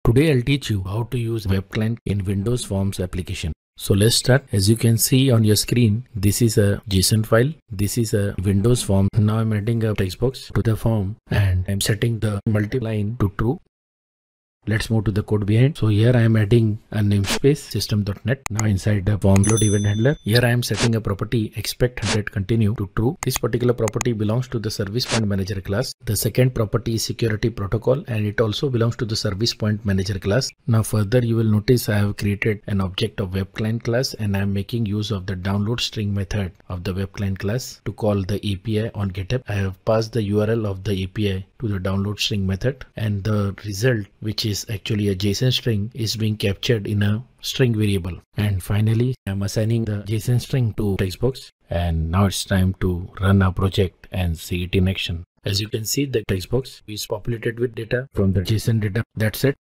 Using WebClient in Windows Forms
Here Mudassar Khan has explained with an example,  how to use WebClient in Windows Forms (WinForms) Application in C#.